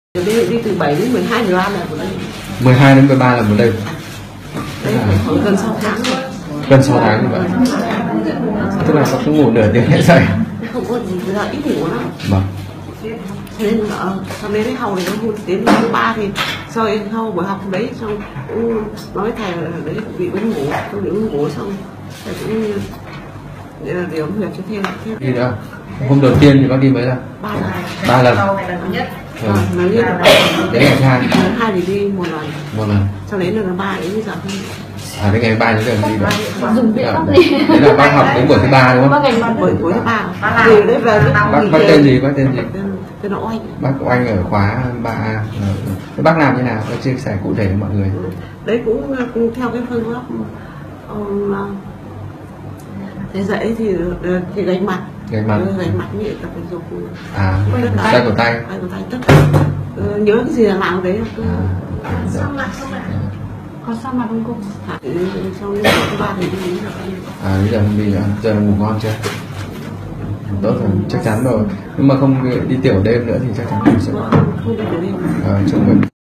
3 NGÀY TRỊ DỨT MẤT NGỦ DO TIỂU ĐÊM
3 NGÀY TRỊ DỨT MẤT NGỦ DO TIỂU ĐÊM.
Cô bị mất ngủ kéo dài 6 tháng nay do một đêm cô phải đi tiểu đến 13 lần. Cô nghe học viên khoá trước mách đi học Diện Chẩn hay lắm về mà tự làm. Thế là cô đến lớp, cũng chẳng biết có học được không nữa!?
Thầy giảng đến đâu cô về thực hành đến đó...Quả nhiên ngày đầu tiên giảm tiểu đêm còn 3 lần, ngày 2 thì còn 1 lần, ngày 3 thì dứt hẳn từ hôm đó đến nay. Giờ thì cô có giấc ngủ ngon giấc rồi. Thật ngoài sức tưởng tượng của cô!
Hãy nghe người trong cuộc làm như thế nào nhé;
Lớp học tại Clb Diện Chẩn Việt do Thầy Vũ Văn Hội giảng dạy